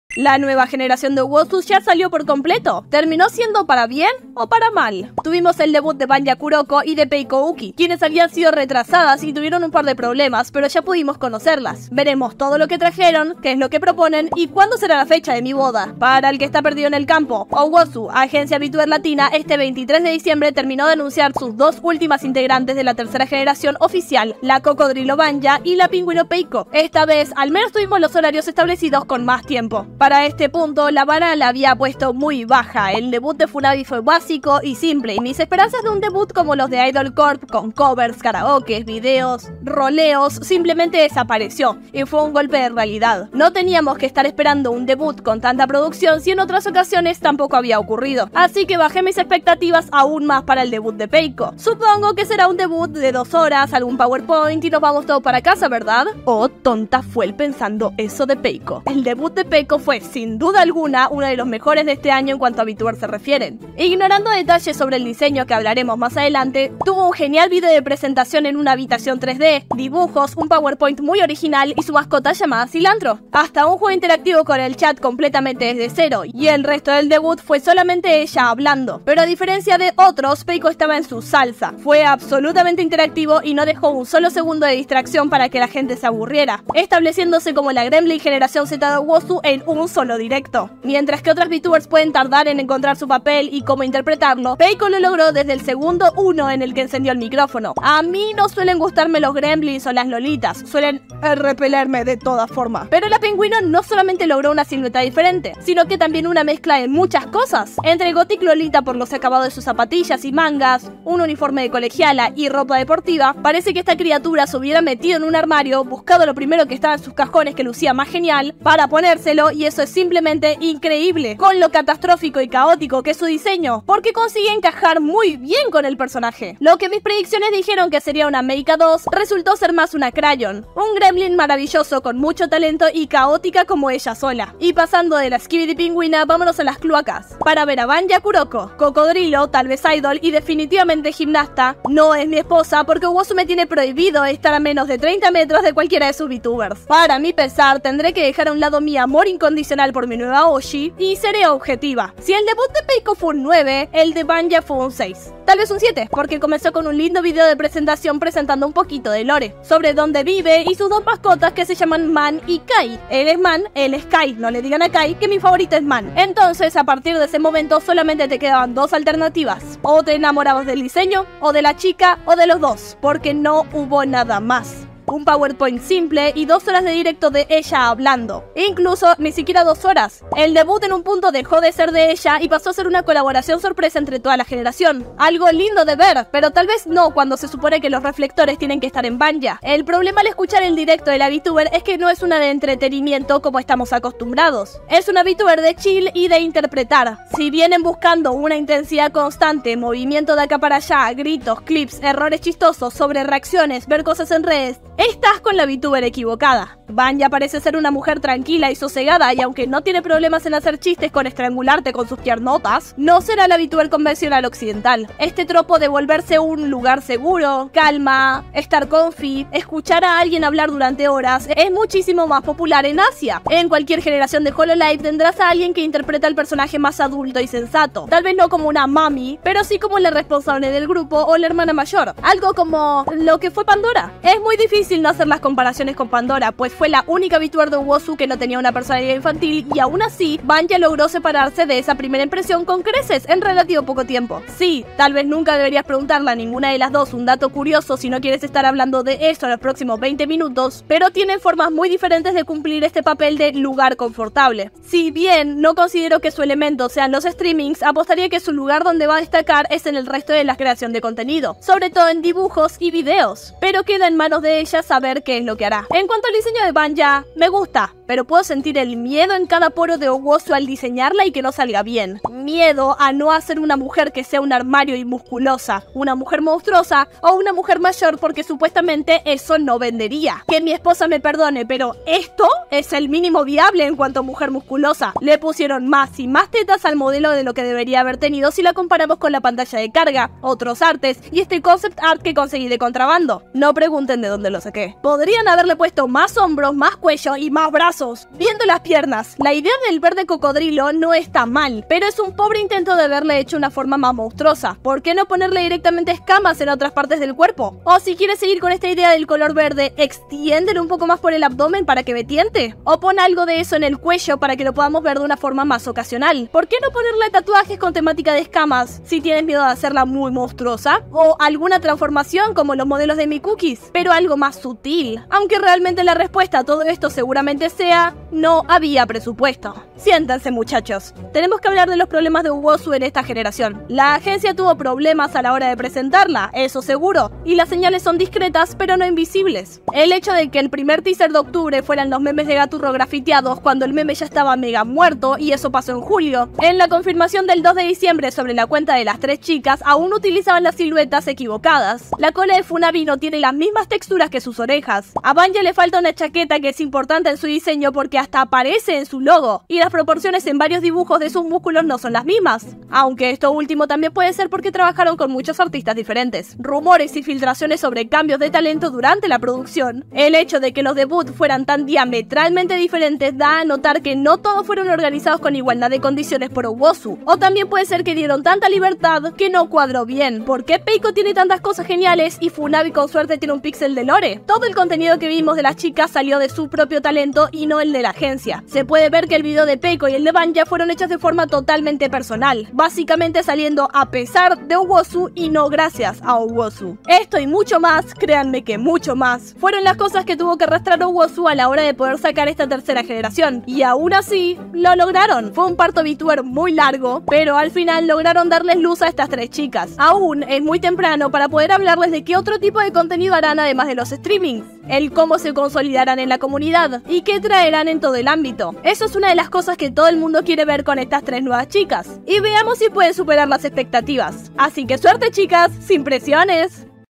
LA NUEVAS VTUBERS DE OWOZU
Al fin terminaron los Debuts en Owozu, pero no todo esta claro aun ¿Por que algunos debuts fueron un 10 y otros un 5? ¿Qué tanto influencia Owozu en sus talentos? Algo esta faltando.